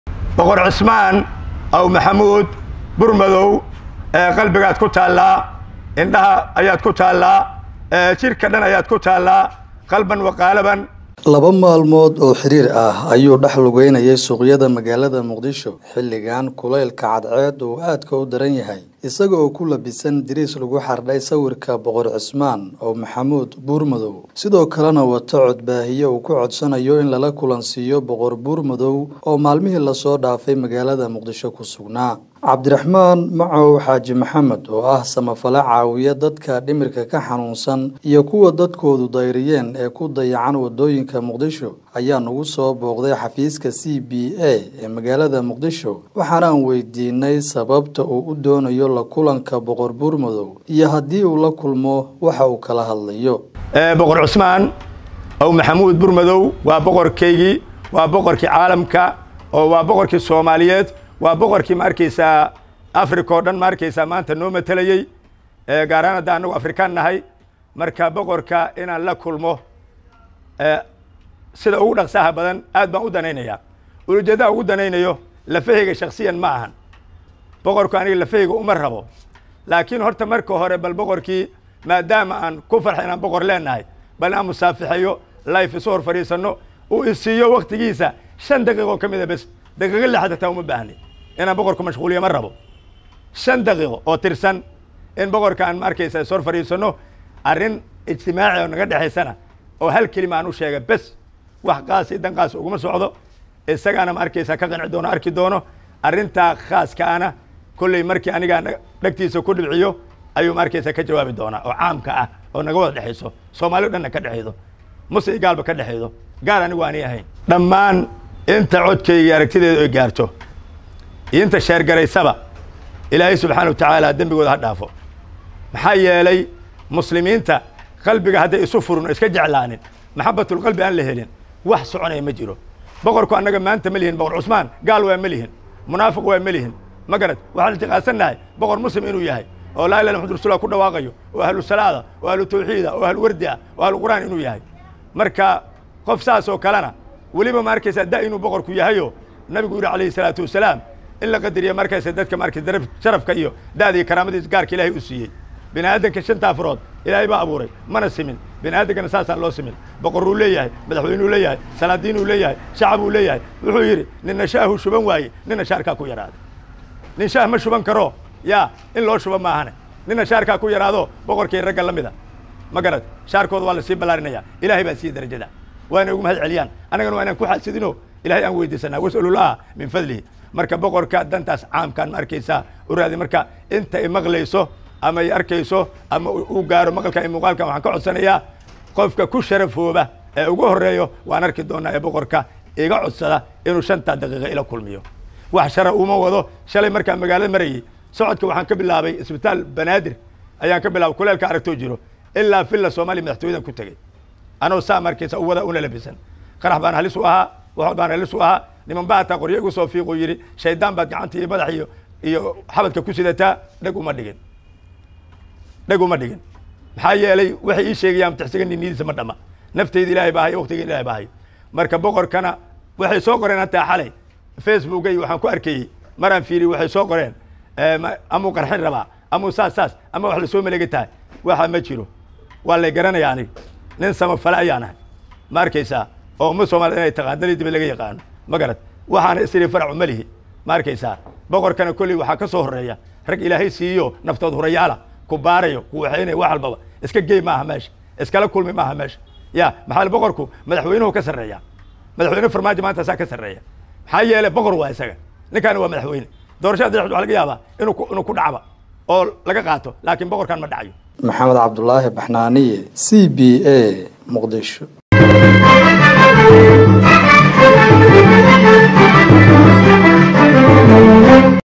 Odey Muqdisho ka baadigoobaya Boqor Buurmadow, si uu ula kulmo 5 daqiiqo.
NIN Oday Ah oo labo maalmood wadooyinka muqdisho u dhax-wareegaya la kulanka Boqor Buurmadow